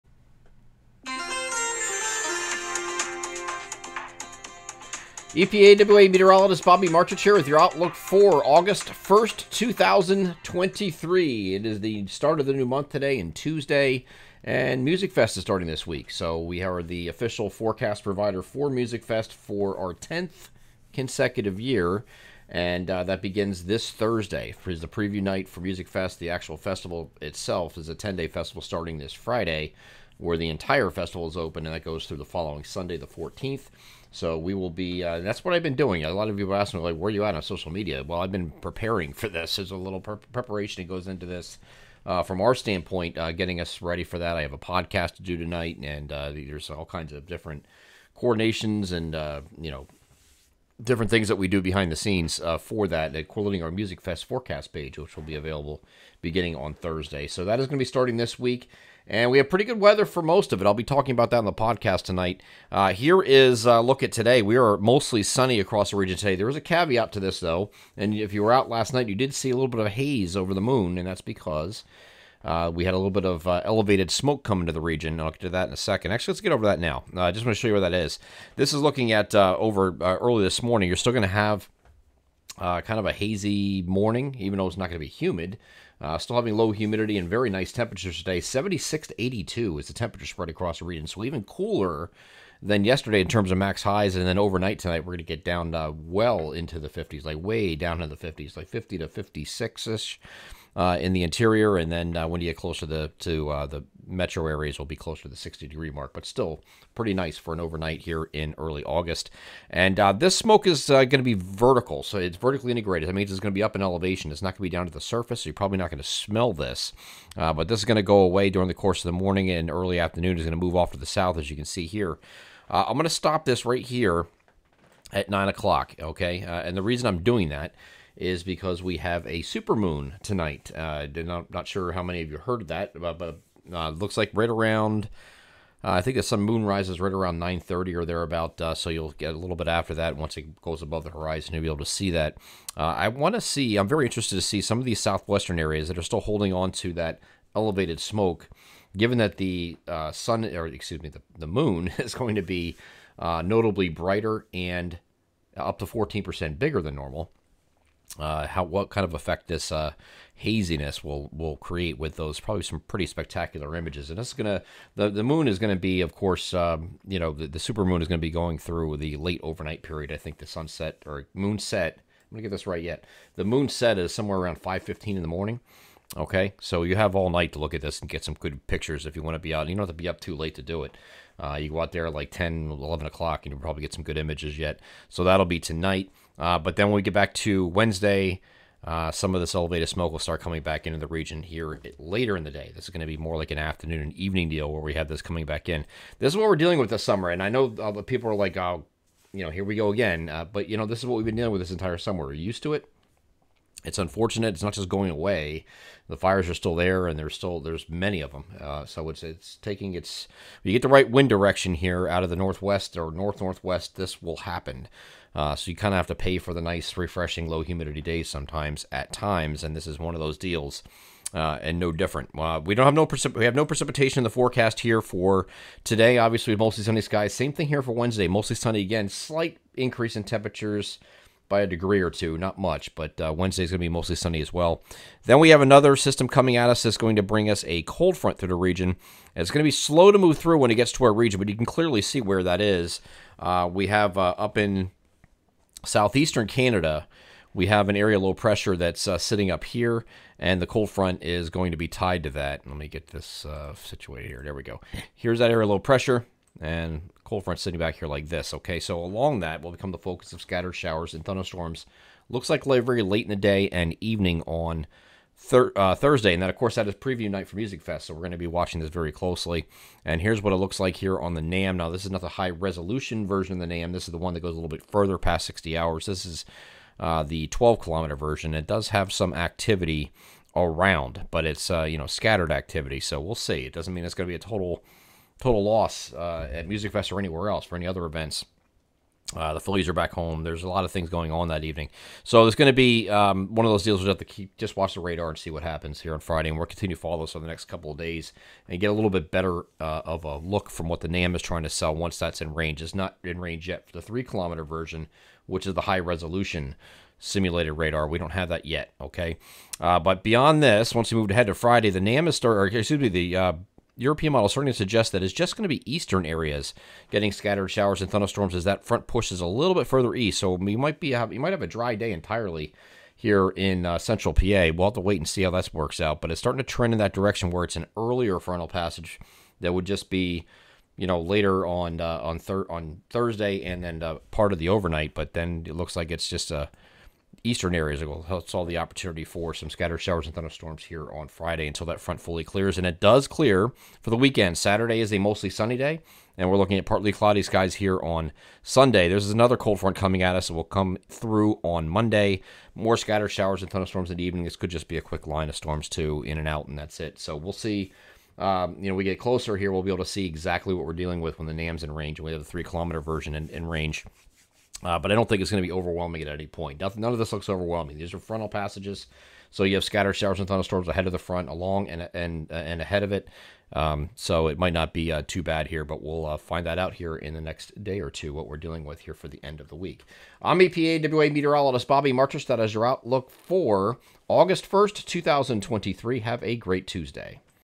Tuesday August 1st, 2023 video forecast
EPAWA's 8/1 and week ahead outlook, covering:
■ Elevated wildfire smoke a factor near term
■ Supermoon tonight (first of two this month)
■ Next chance for rain/storms in the week ahead